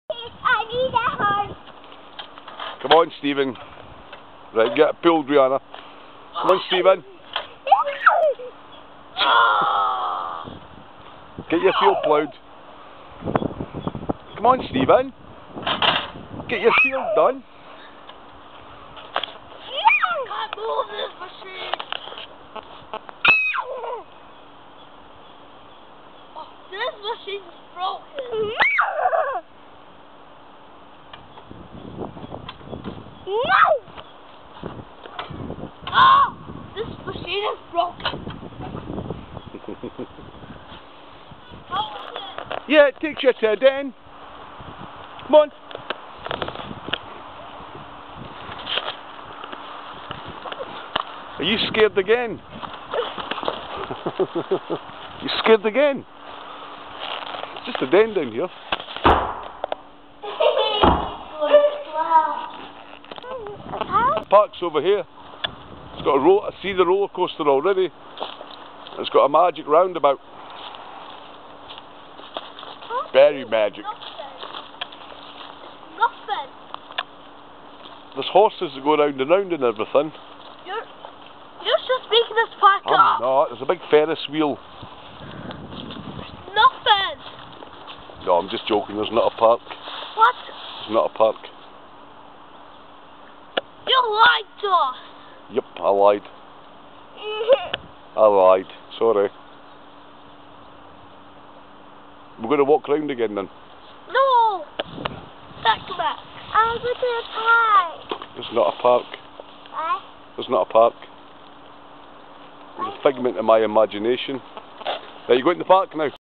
Crombie Park Bear Chase
Steven And Rhianna Think They Are Getting Chased By Bears All The Time at Crombie Park.